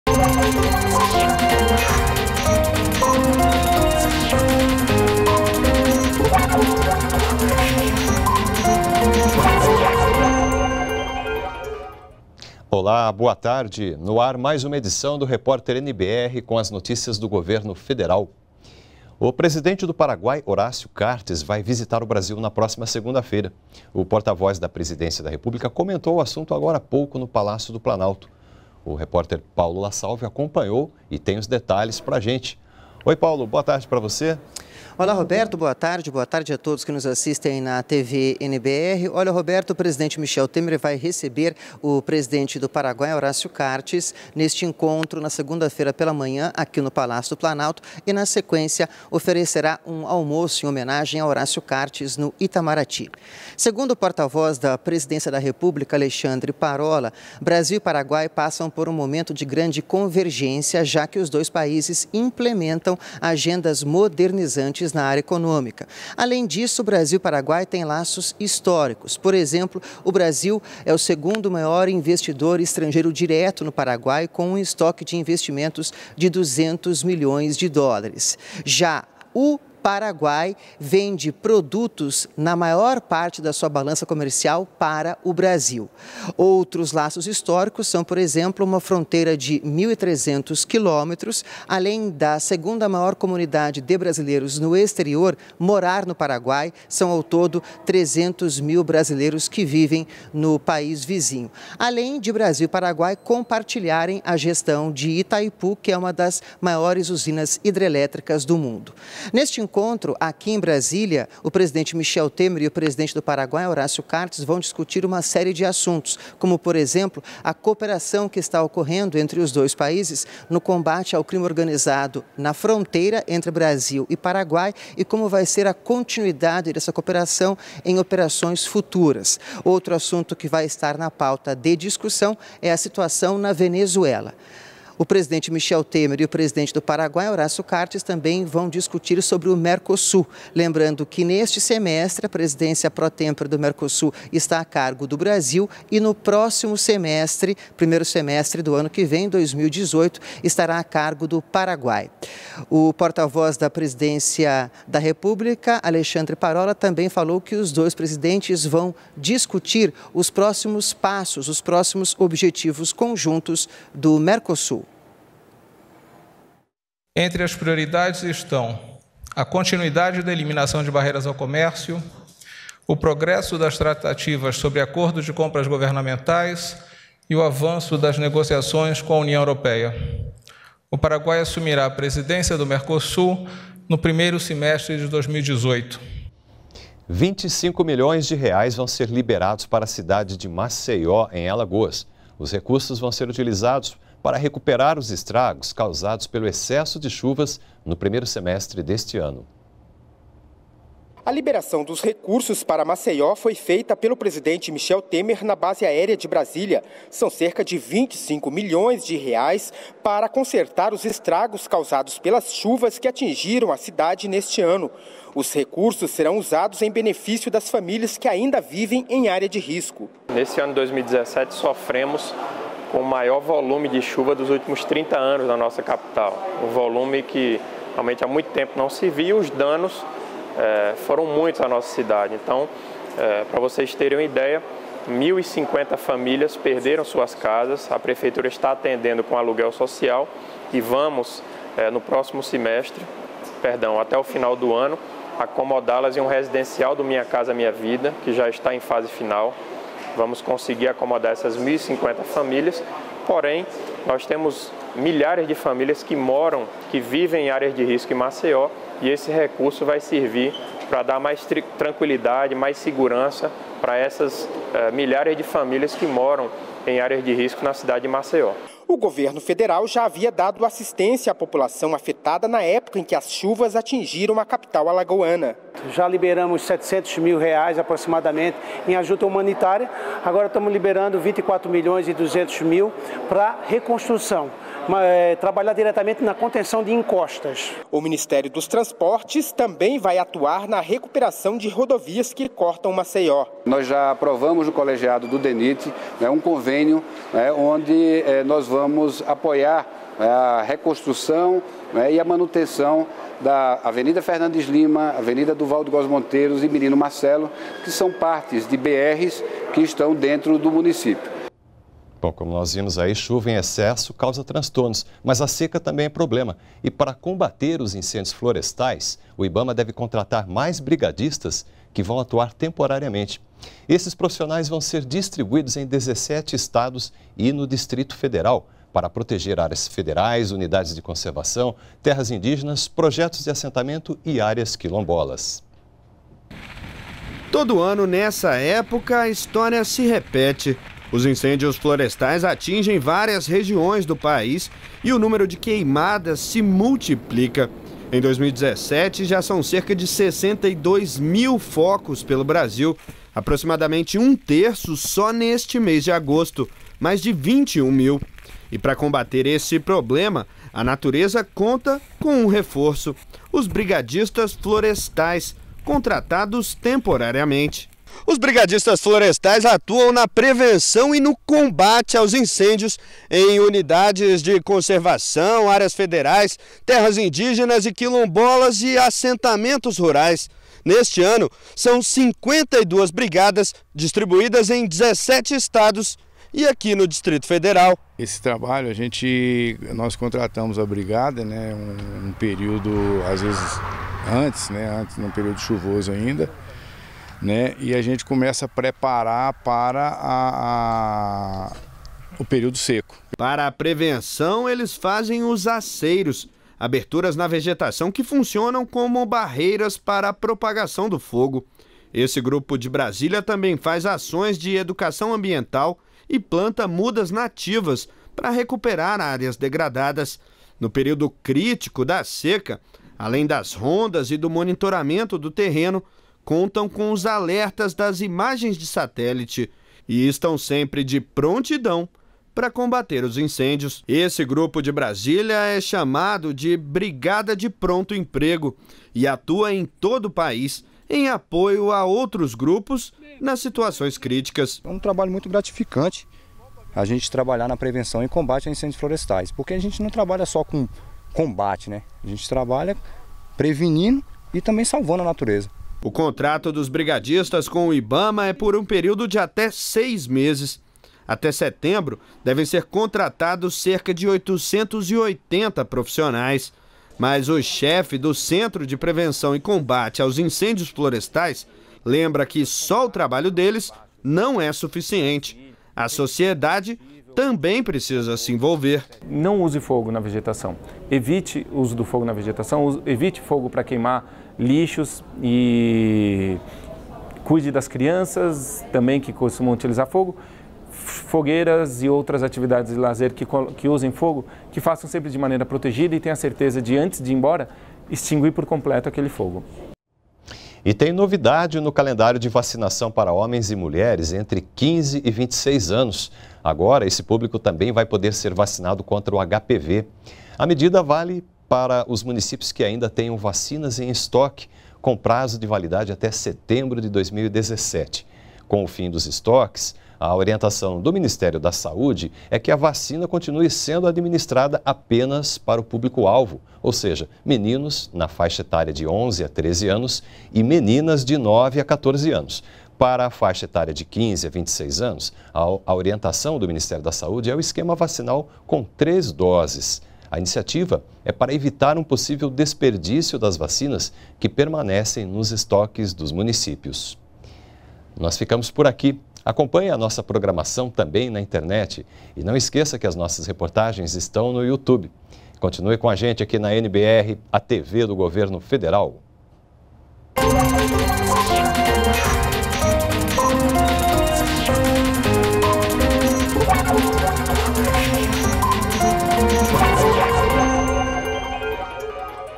Brasil receberá o presidente do Paraguai na próxima segunda-feira
REPÓRTER NBR 16H - 18.08.17: O presidente do Paraguai estará no Brasil na próxima segunda-feira (21) para discutir cooperação entre os países. Cerca de R$ 25 milhões serão enviados para Maceió, capital, de Alagoas; os recursos serão usados para reparar danos causados pelas fortes chuvas que atingiram a cidade. Ibama vai contratar mais brigadistas para atuarem temporariamente no combate às queimadas.